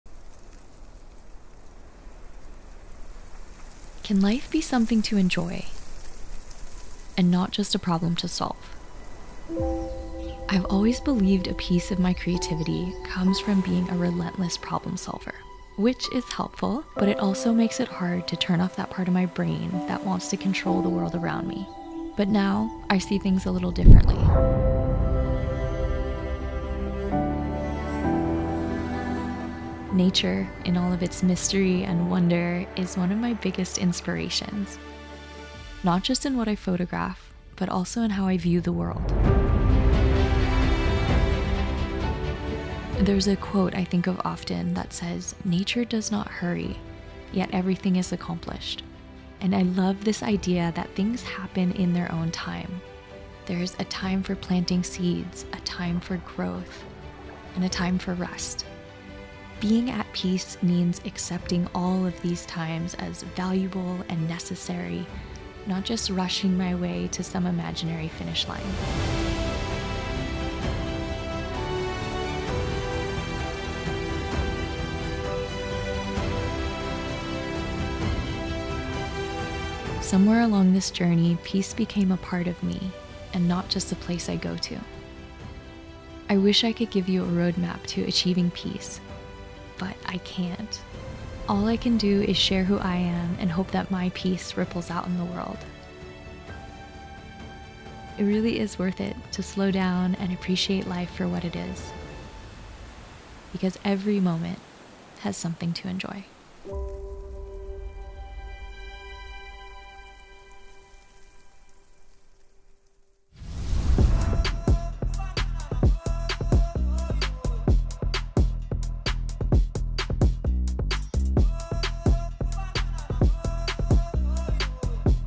A Film on Chelsea Kauai (@ChelseaKauai for Made Wild Recess)
A cinematic short film on Chelsea Yamase (@ChelseaKauai) as she talks about peace.

Chelsea Yamase is a Hawaii born creative. Her work promotes environmental advocacy, mindful productivity, and finding joy in everyday moments.

This is Episode Five of our original five film series called "Made Wild Recess - Season 2" featuring creators Sam Newton, Chelsea Yamase, Liv Swearingen, Chris Balladarez, and Jacob Ferrufino.

Shot handheld on Red Komodo with Canon glass.
Director of Photography: Josiah Selvig
Producer/Photographer: Sarah Selvig
Talent:  @ChelseaKauai